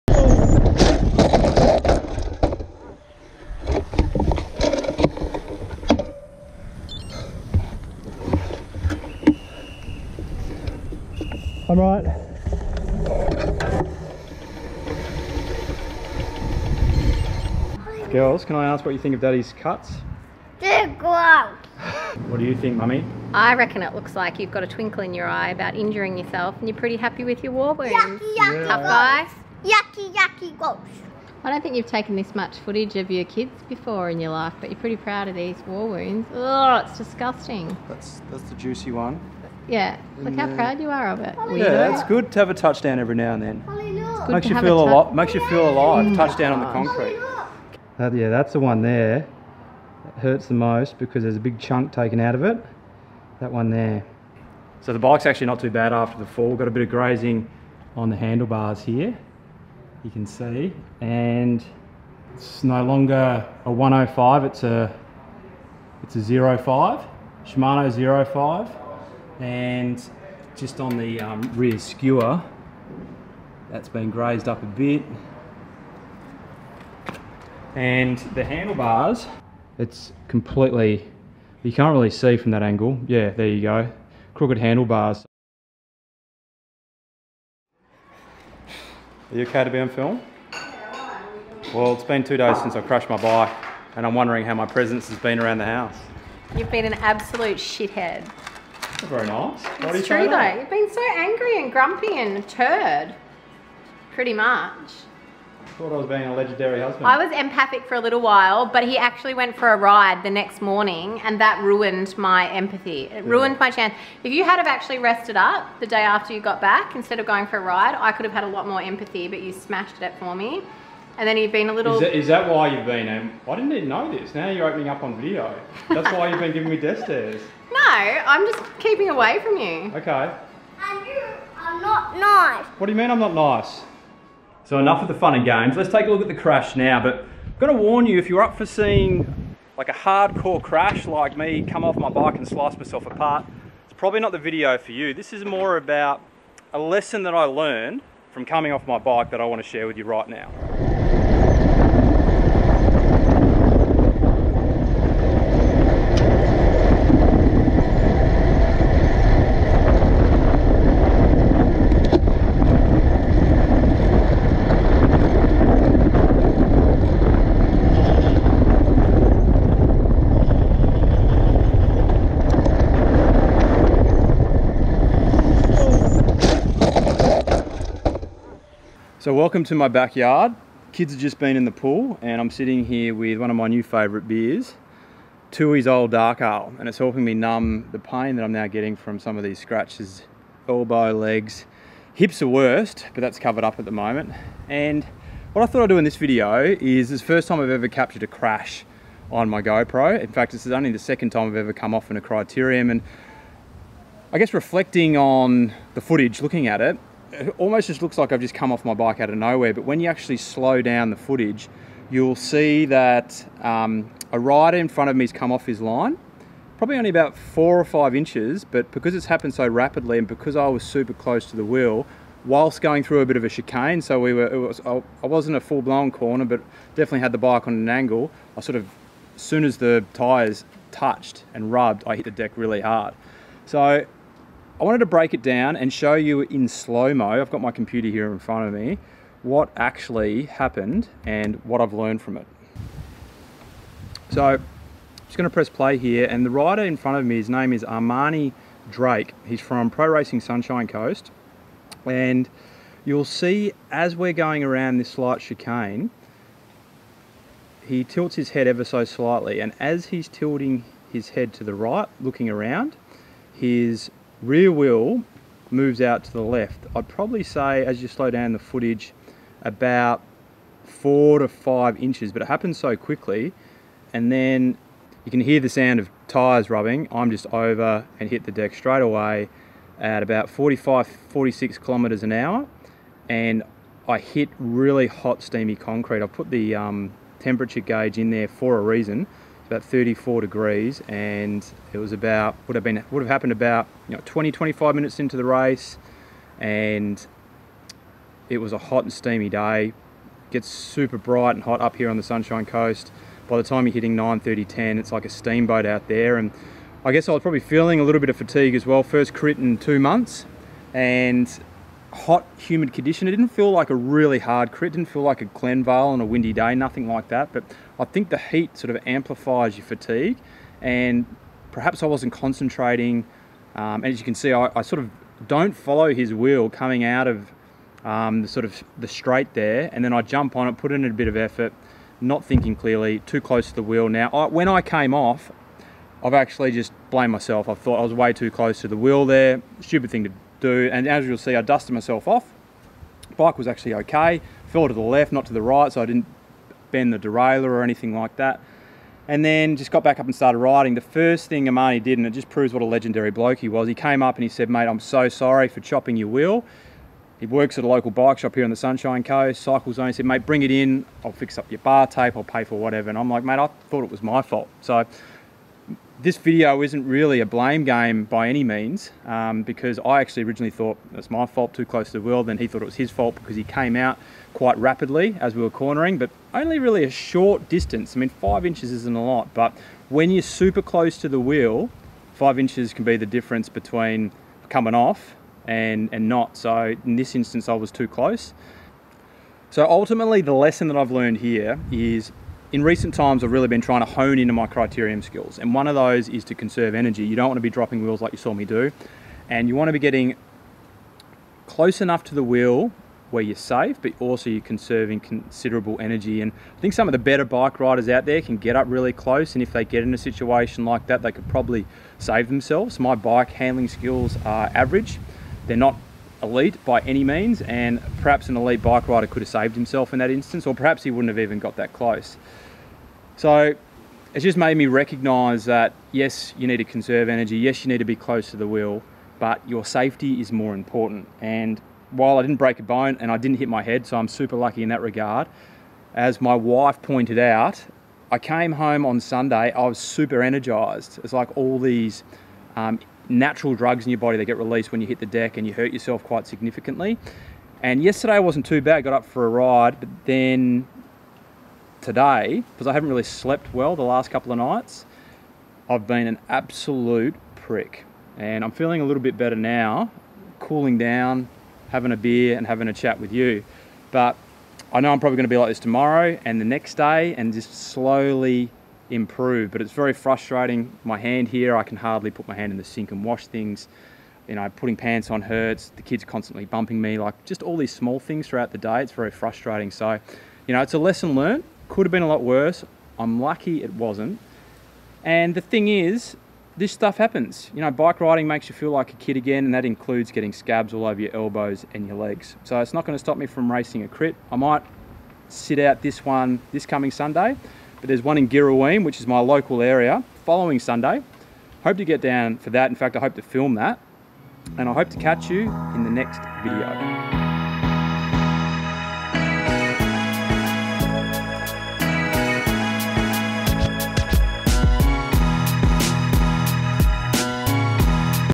Crashing Hard in a Criterium & New Lessons
In this video, I will share with you a cycling crash I recently had at my local criterium race. Riding the Specialized Allez Sprint with a Shimano 105 groupset and Enve 6.7 wheels, I come off my road bike at around 45 km per hour. This crash is a new lesson. While getting close to the wheel in front of you enables you to converse energy and is a common criterium tip, my lesson here is that safety comes first. In this crash, I am lucky not to hit my head or break any bones!